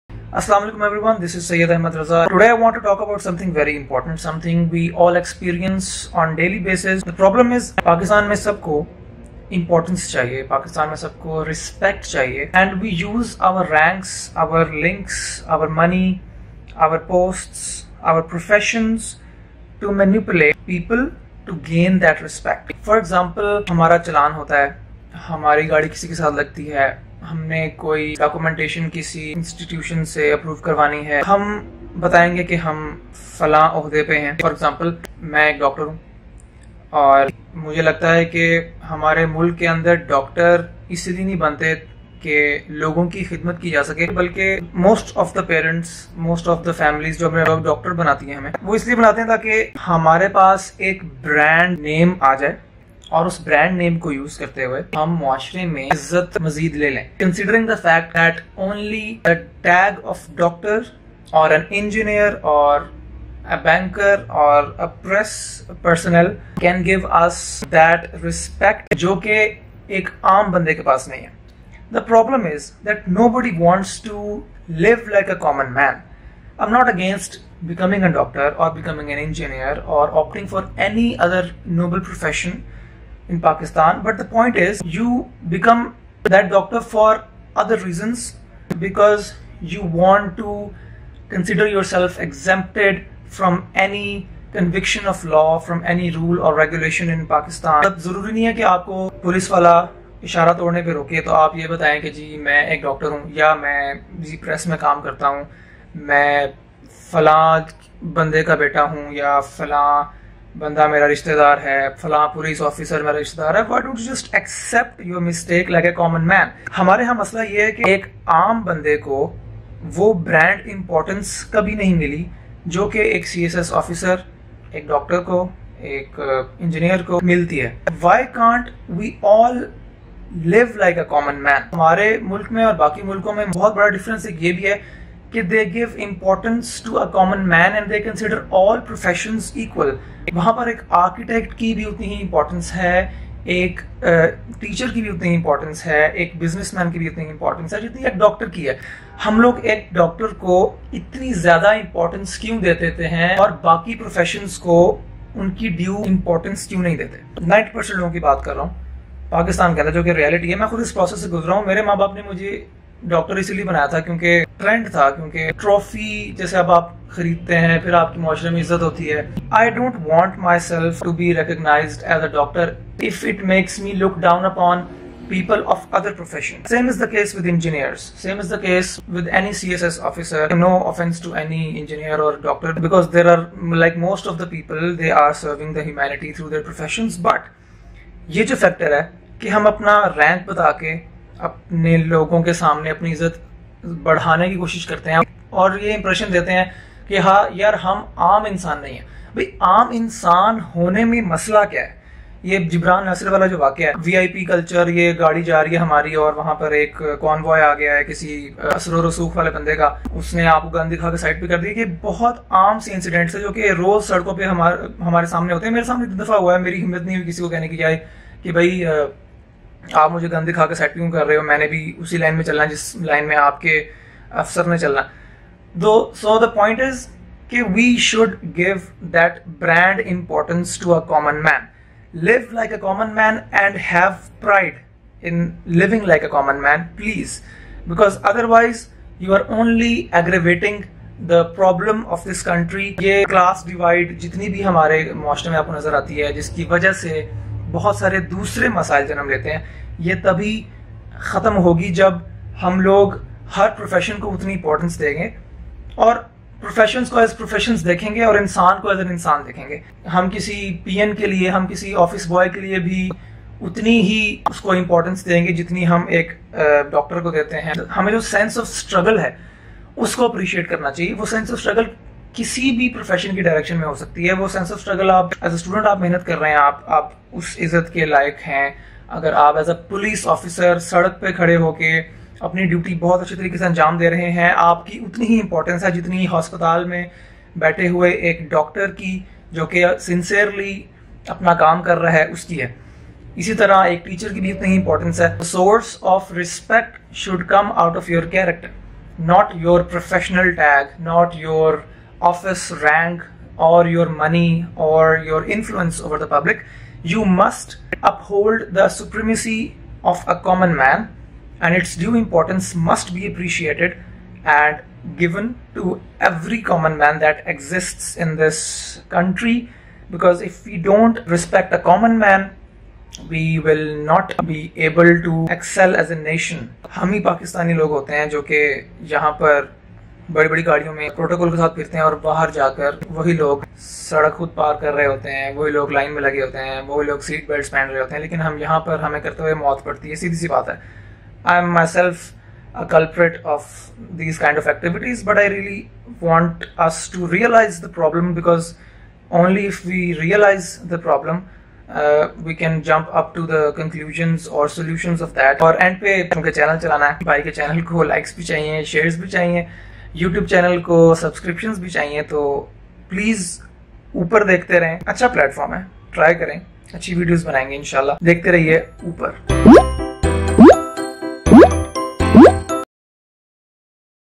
Why can't we live like a common man?
What does everyone in Pakistan want to get special treatment because of their ranks, posts, money, etc.?
What is wrong with living like a common man?